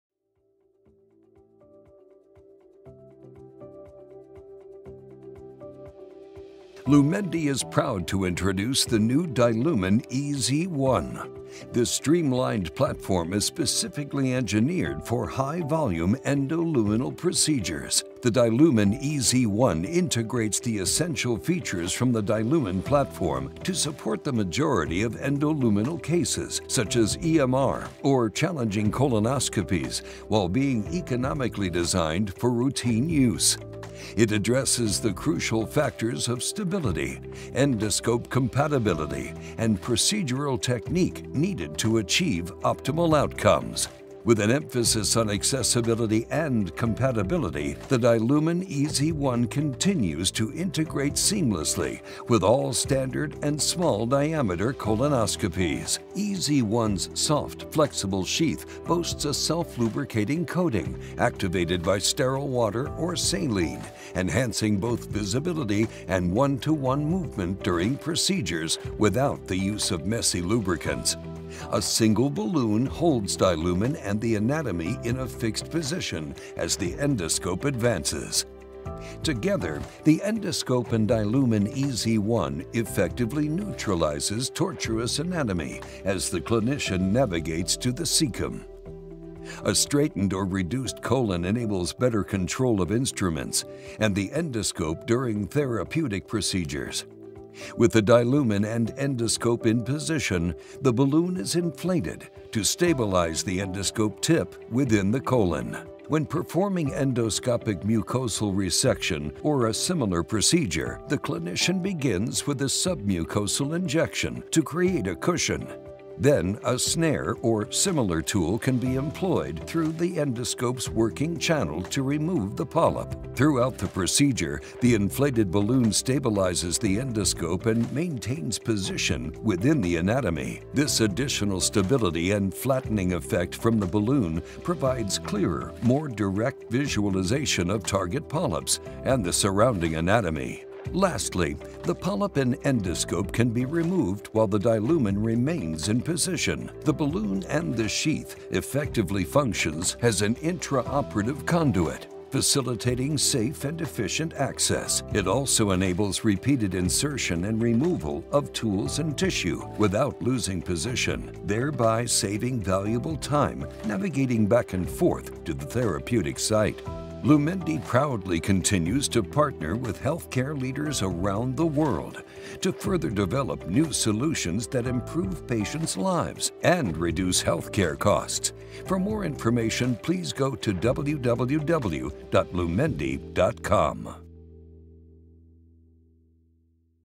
EZ¹ provides more options for challenging procedures
Confident Navigation
Simplified, one-balloon design for improved endoscopic stability
Atraumatic conduit enables safe and efficient repeated access or removals of tools and tissue

Precise control
Helps enable colon shortening, to straighten tortuous anatomy for easier, faster access and improved endoluminal tool usage

Enhanced visualization
Gentle endoluminal anchoring via the balloon helps reveal hidden tissue planes and polyps